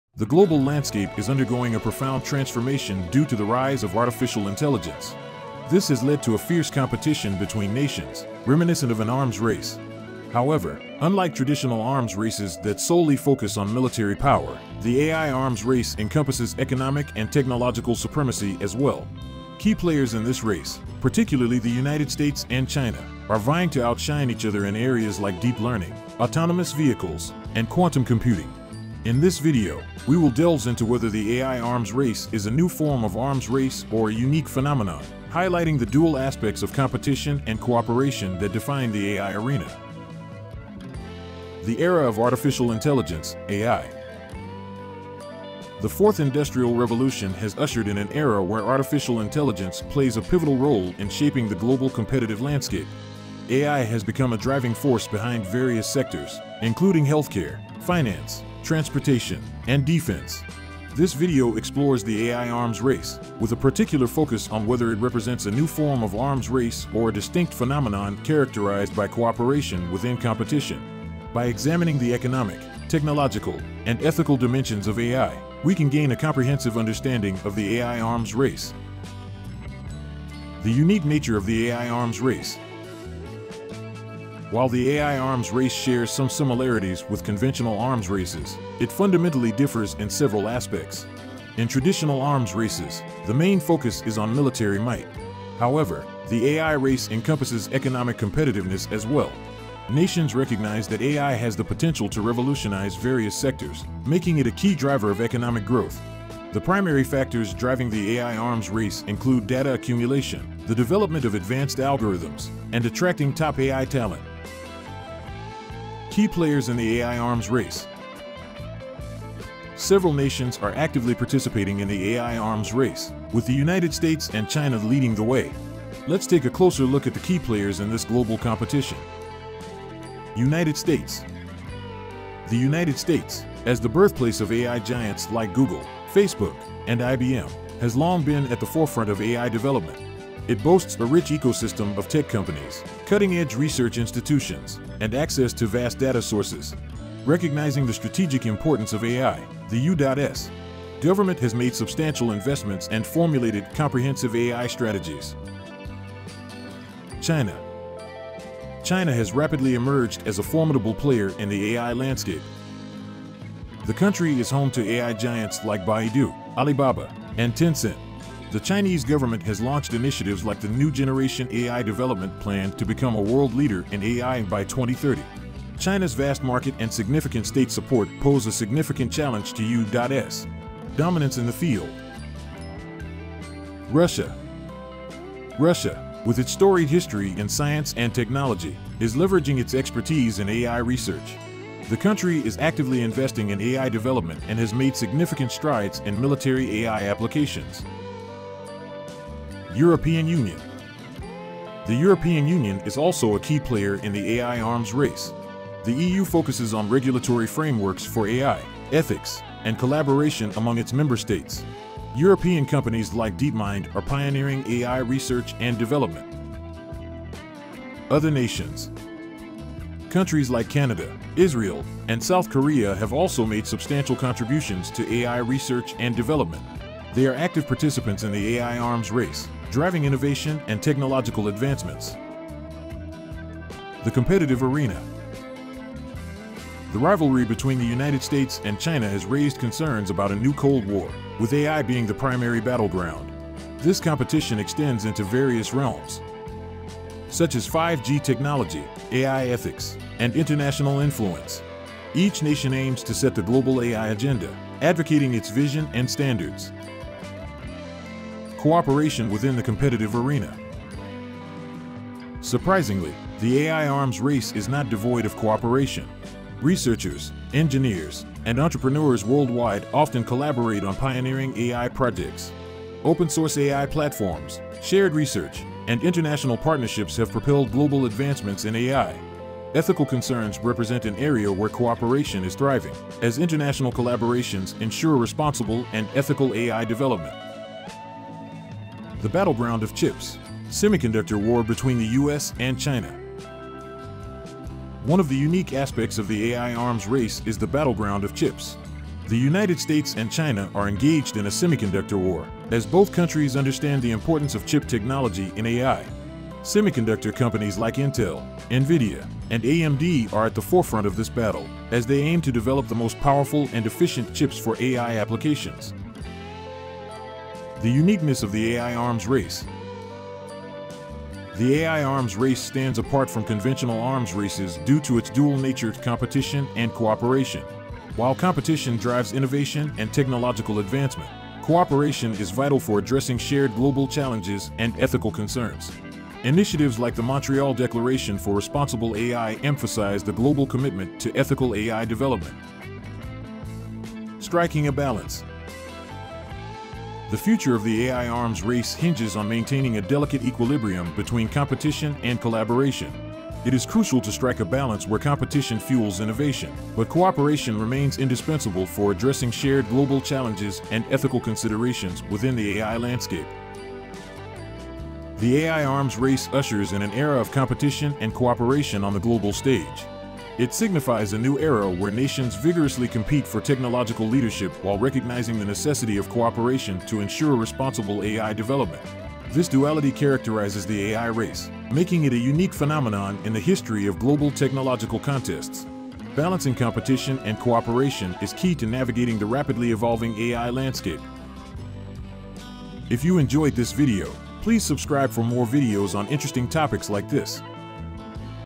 AI War Between Nations: A New Arms Race or Cooperation?
In this video, we'll be discussing the possible outcomes of the AI War Between Nations: A News Arm Race or Cooperation?

The AI War Between Nations is a future conflict that has the potential to change the world as we know it. In this video, we'll discuss the possible outcomes of this war, and what it will mean for humanity. Are we headed for a new arms race, or are we prepared to cooperate? Let's find out in this video!